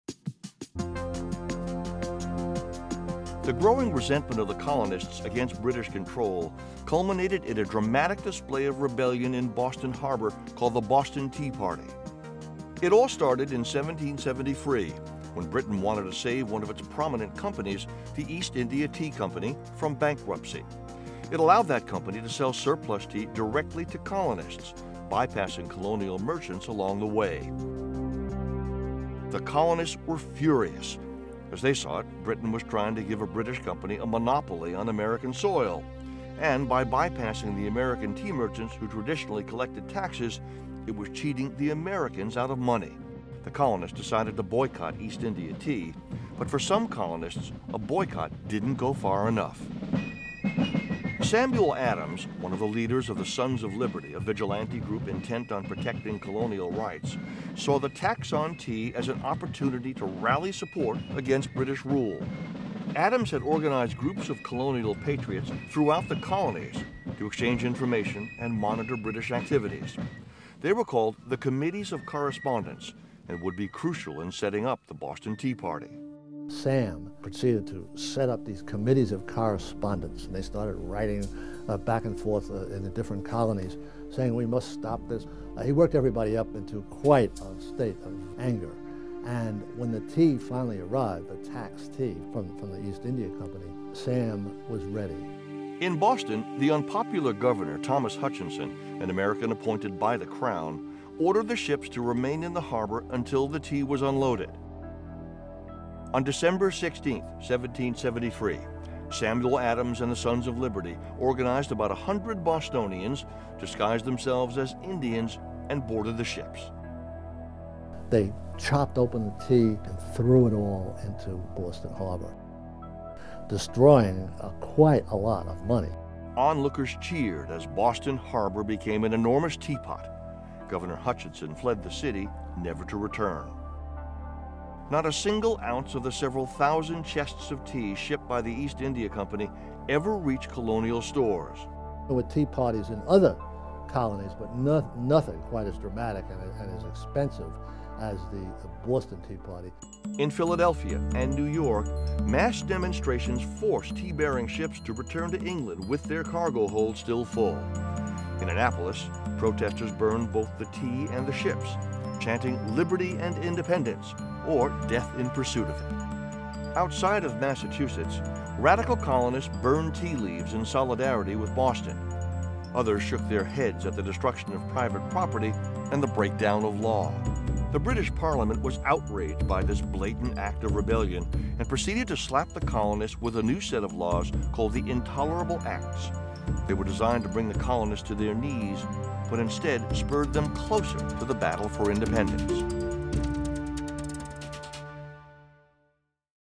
The Boston Tea Party
When several attempts to tax the American colonies fail, Britain attempts a compromise by taxing tea. Colonial radicals led by Samuel Adams of Boston are incensed and dump the British tea into Boston Harbor.